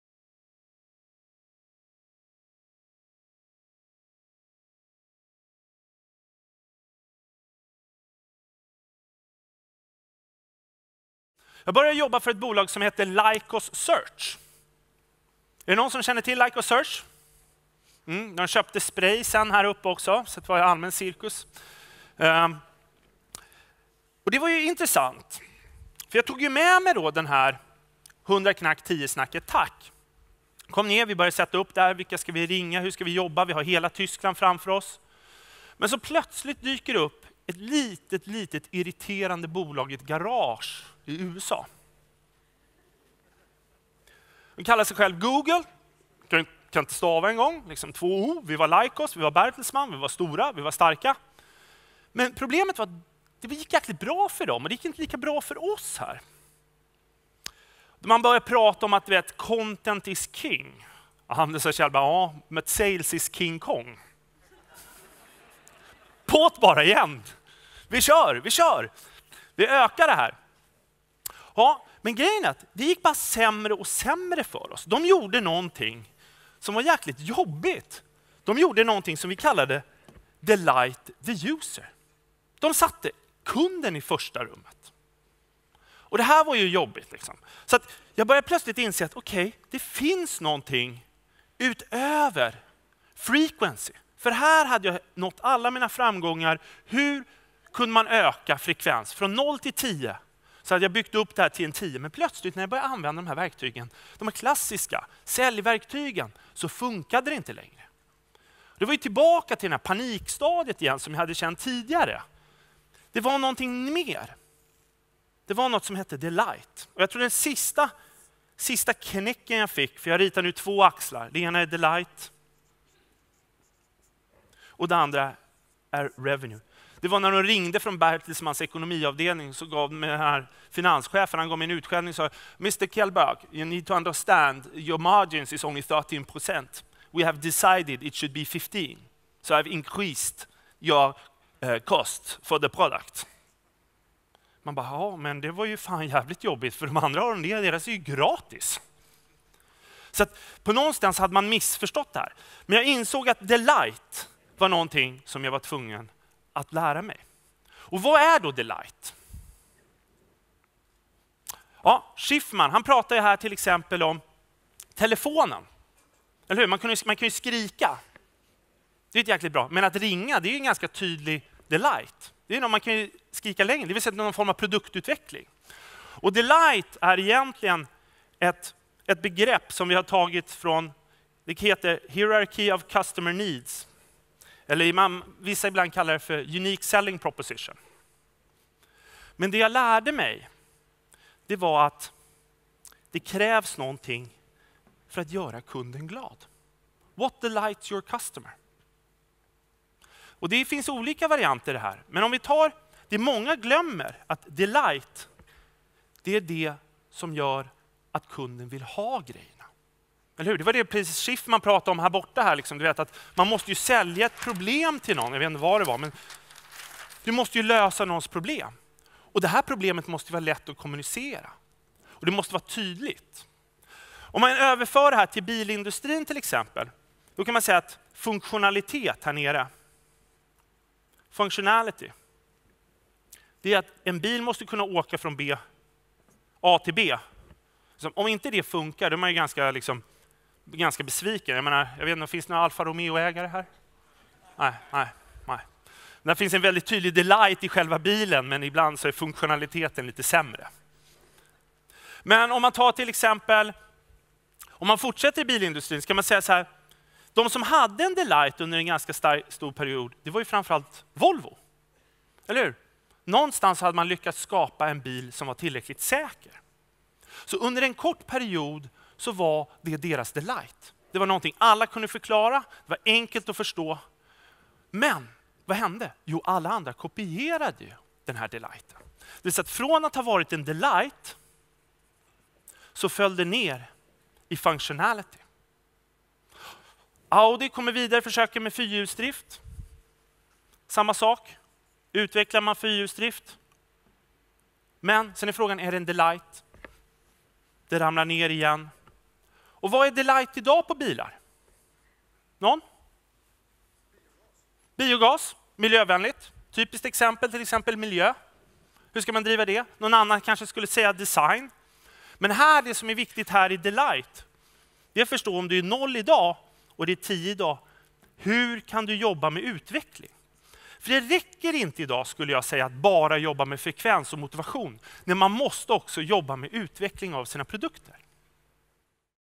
Sales Management 2.0 - Jonas Kjellberg.mov
Jonas Kjellberg på Sales Management 2.0. Öka försäljningen genom "delight" och ett bättre samspel mellan försäljning och produktutveckling.